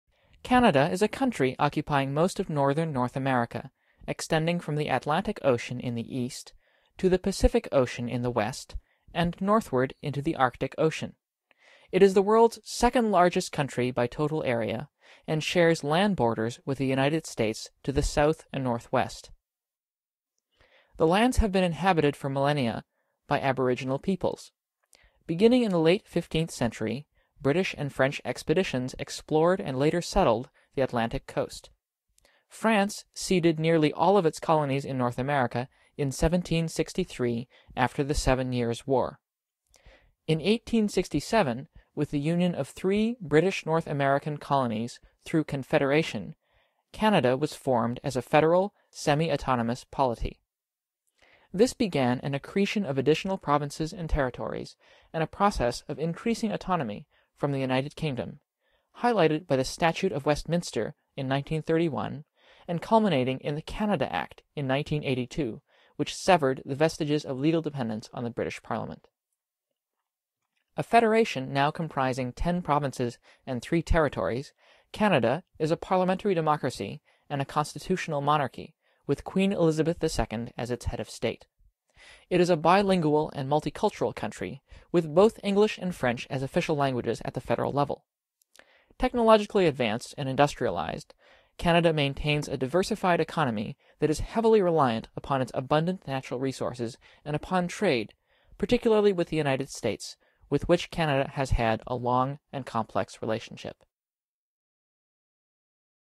Canada: Country Profile
Canada is a country occupying most of northern North America, extending from the Atlantic Ocean in the east to the Pacific Ocean in the west and northward into the Arctic Ocean. It is the world's second largest country by total area, and shares land borders with the United States to the south and northwest.

The lands have been inhabited for millennia by aboriginal peoples. Beginning in the late 15th century, British and French expeditions explored and later settled the Atlantic coast. France ceded nearly all of its colonies in North America in 1763 after the Seven Years War. In 1867, with the union of three British North American colonies through Confederation, Canada was formed as a federal, semi-autonomous polity. This began an accretion of additional provinces and territories and a process of increasing autonomy from the United Kingdom, highlighted by the Statute of Westminster in 1931 and culminating in the Canada Act in 1982 which severed the vestiges of legal dependence on the British parliament.

A federation now comprising ten provinces and three territories, Canada is a parliamentary democracy and a constitutional monarchy with Queen Elizabeth II as its head of state. It is a bilingual and multicultural country, with both English and French as official languages at the federal level. Technologically advanced and industrialized, Canada maintains a diversified economy that is heavily reliant upon its abundant natural resources and upon trade—particularly with the United States, with which Canada has had a long and complex relationship.

Painting: Detail of Raphael's Sistine Madonna
Photo by: Carulmare